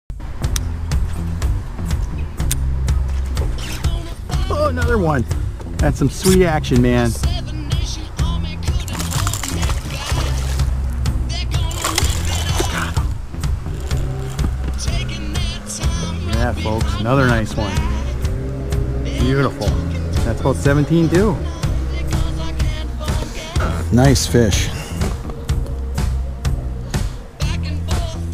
River Retaining Wall Pays off BIG!!! [SWEET ACTION]
A retaining wall on the Milwaukee River was loaded with smallmouth bass and this was the 3rd one I landed off the same wall!
The CL8Bait Might Mouse was on fire and most of the fish I caught slammed it with reckless abandon! Who needs NDYakAngler!